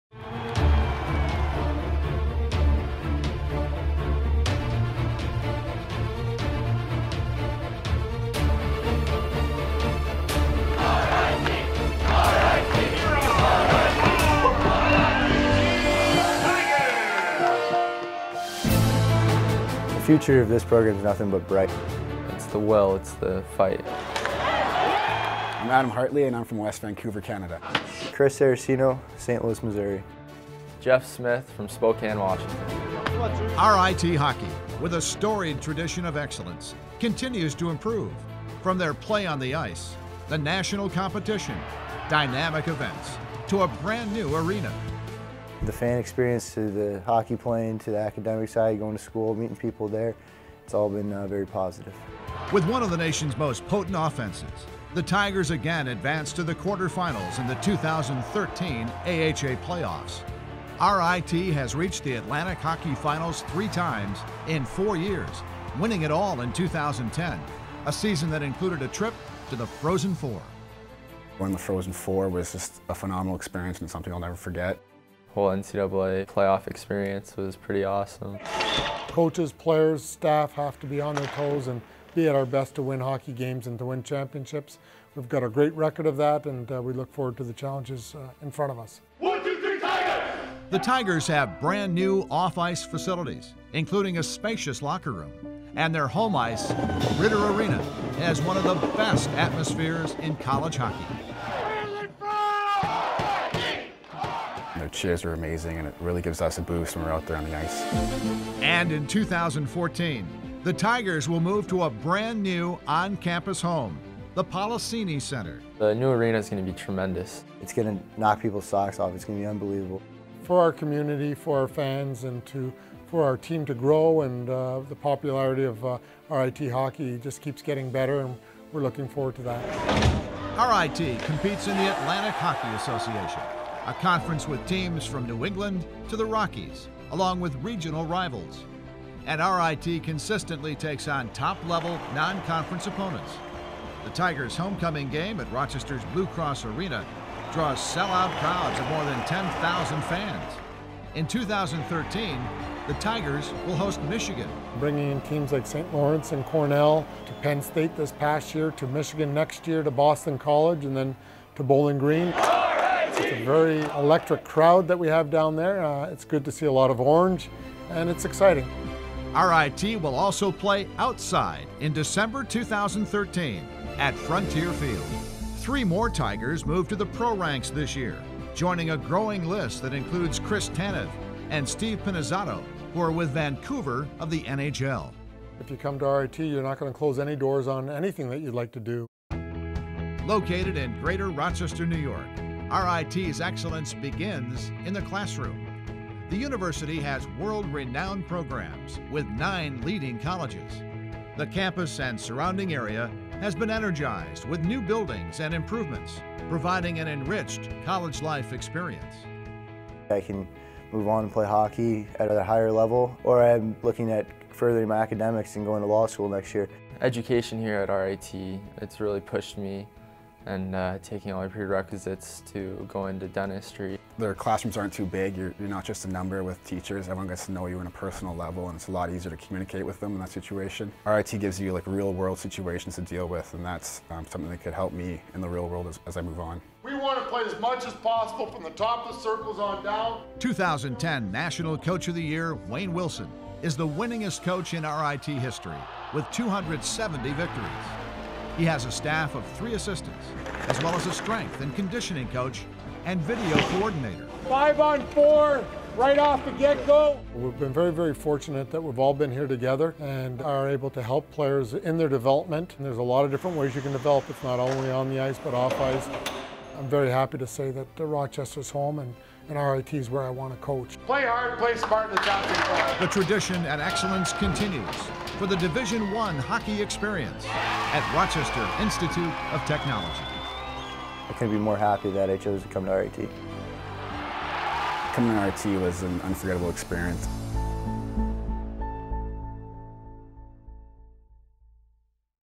RIT Men's Hockey Team Video 2012-2013
RIT Hockey, with a storied tradition of excellence, continues to improve; from the play on the ice, the national competition, dynamic events to a brand new arena.
With one of the nation's most potent offenses the Tigers again advanced to the quarterfinals in the 2013 AHA playoffs.    RIT has reached the Atlantic Hockey Finals three times, winning it all in 2010, a season that included a trip to the NCAA Frozen Four.